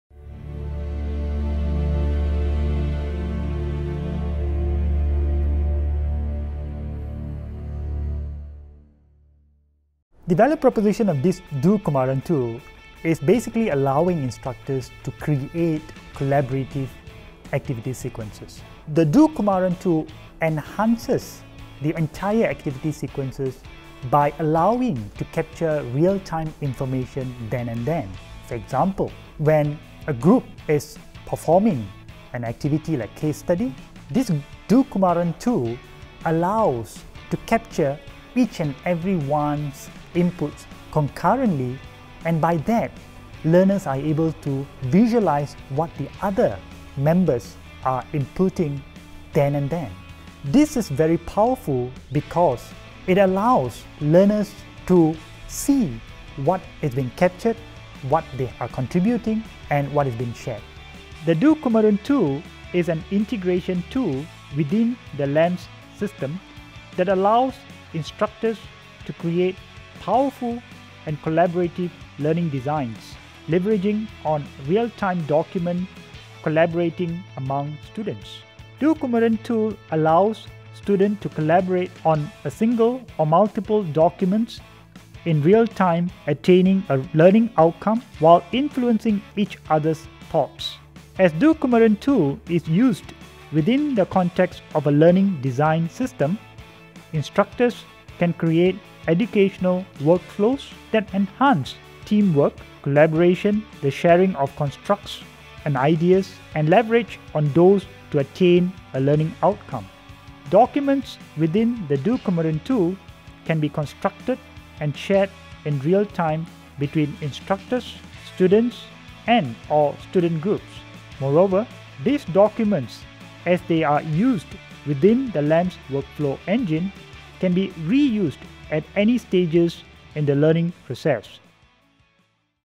Value Proposition of dKT - ‘doKumaran Tool’
The ‘doKumaran’ tool is an integration tool within the LAMS system that allows teachers to create powerful and collaborative designs leveraging on real-time document collaborating among students.  It allows students to collaborate on single or multiple documents in real-time attaining a learning outcome while influencing each other’s thoughts. As ‘doKumaran’ tool is used within the context of a lesson design system (LAMS), teachers can create educational workflows that enhance teamwork, collaboration, the sharing of constructs and ideas and leverage on those to attain a learning outcome. Documents within the ‘doKumaran’ tool can be constructed and shared in real-time between teachers, students and/or student groups. Moreover, these documents, as they are uses within the LAMS workflow engine can be reused at any stages in the learning process. Most importantly, and perhaps one of the most important features of the ‘doKumaran’ tool is that it allows to see how ideas or constructs within each document have been created, how they were contributed and development within a given timeline.

The flexibility to add-on and edit the activity sequences with the functionality of “do Kumaran” tool to adopt to the differing learning setting, environment and context emerges as unique characteristic of the system. The exclusive features are basically the 5 activity sequences that are created with the incorporation of the functionality feature of ‘doKumaran Tool’ within the LAMs. Although by and large, there could be real-time online tools, none of these technology platforms offer pre-designed activity sequences template for users to operate in varying collaborative real-time learning setting.